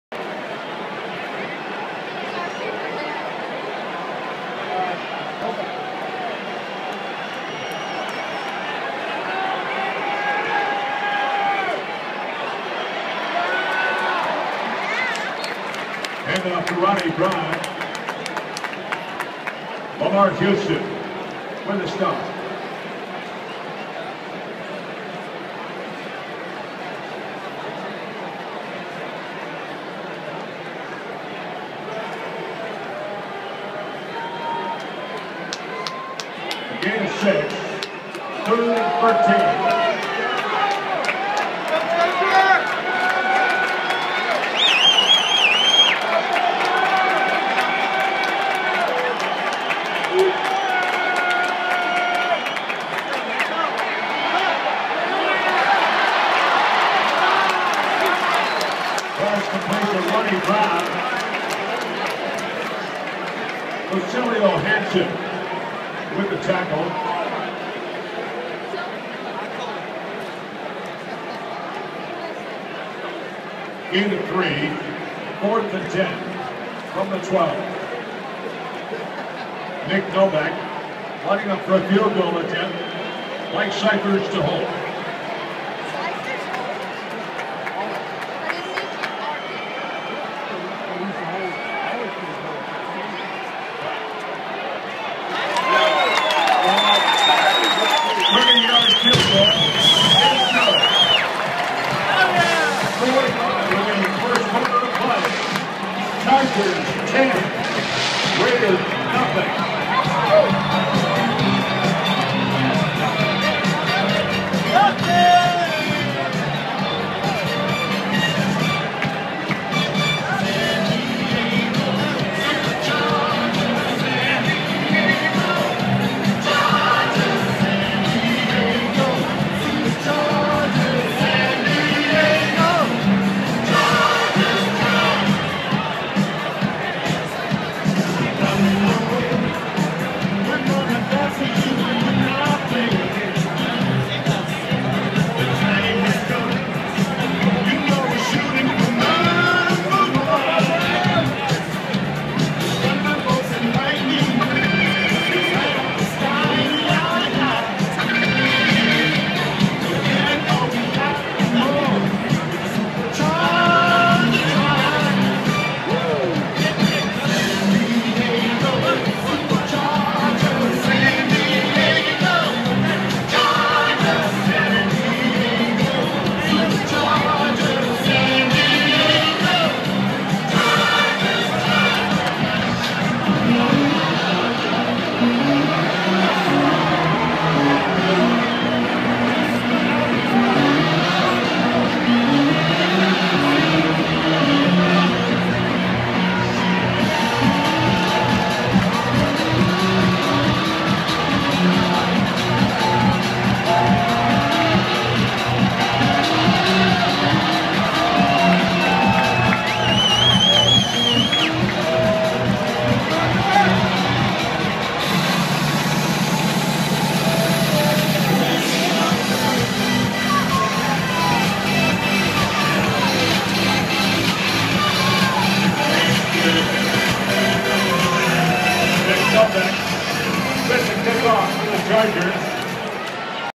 Chargers vs Raiders - 30 Dec 2012
Nick Novak kicks a FG to increase SD's lead to 10-0 early in the game. Chargers eventually win the game, 24-21 to finish 7-9 for the 2012 season.